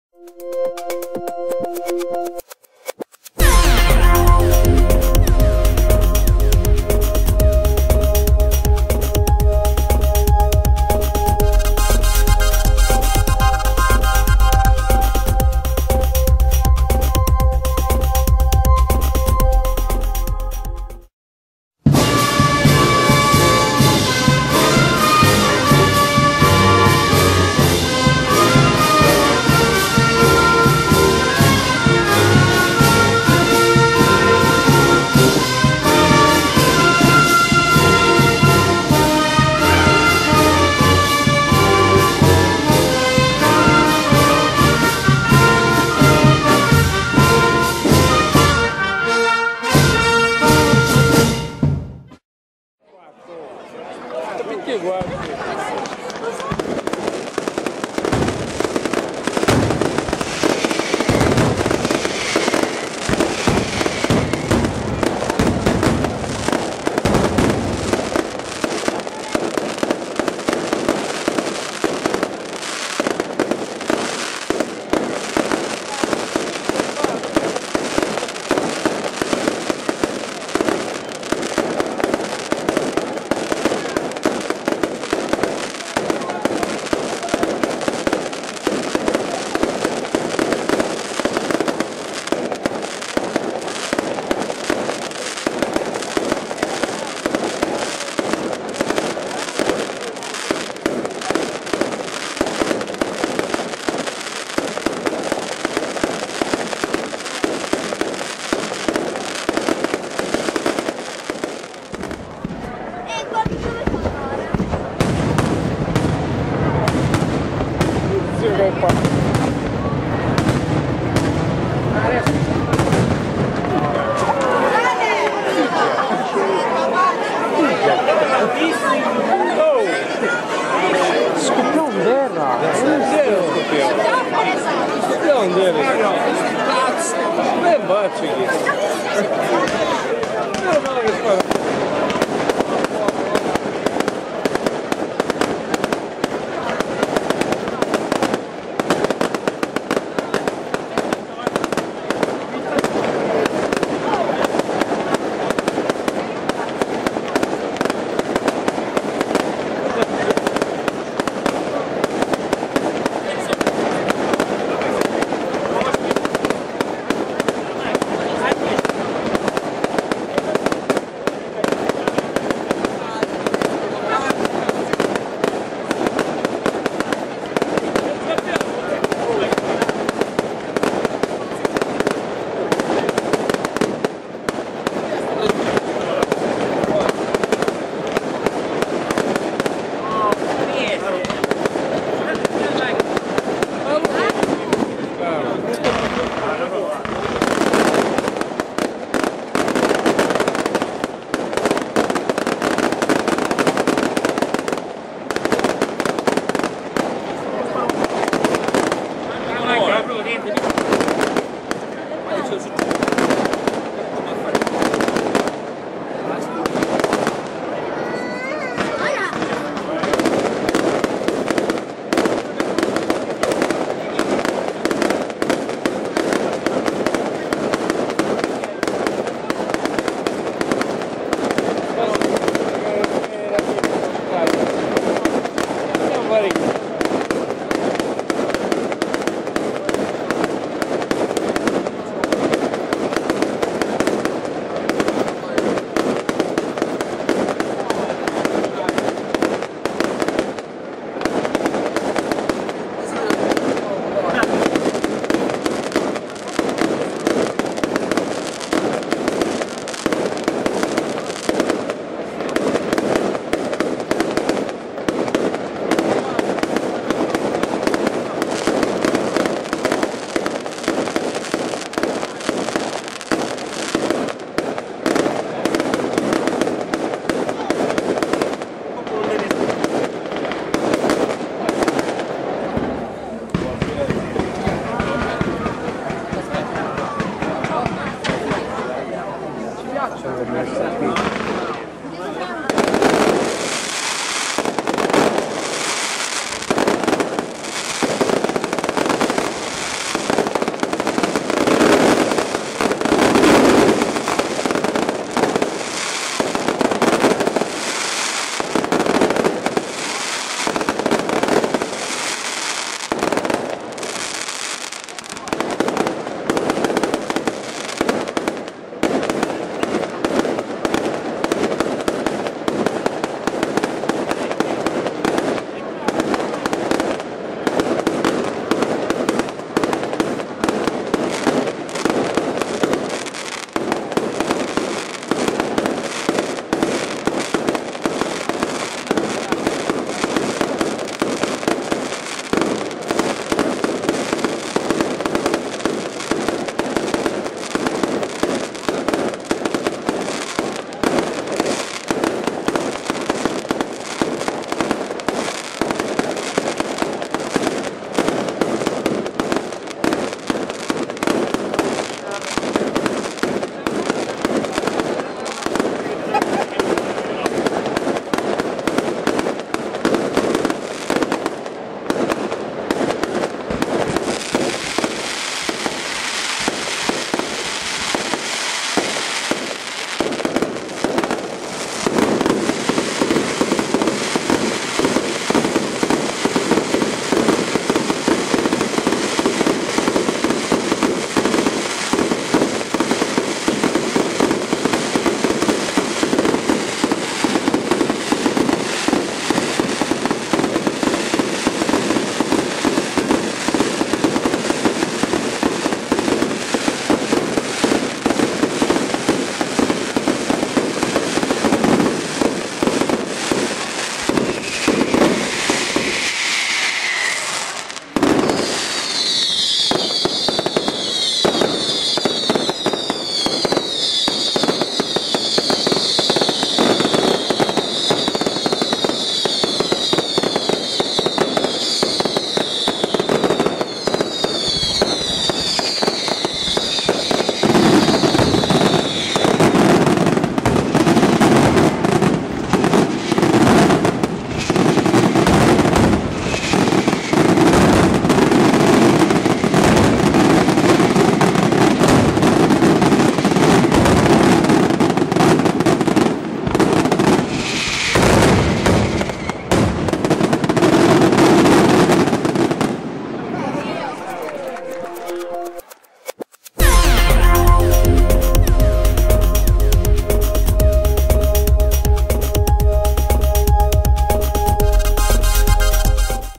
SAN GIOVANNI MONTEBELLO (Catania) - Pirotecnica TORRISI (2015)
SAN GIOVANNI MONTEBELLO (Catania) - 27 Giugno 2015
Festeggiamenti in onore di San Giovanni Battista

Spettacoli pirotecnici "Partito Artigiani" a cura di
Pirotecnica TS di Salvatore Torrisi da Santa Venerina (Ct)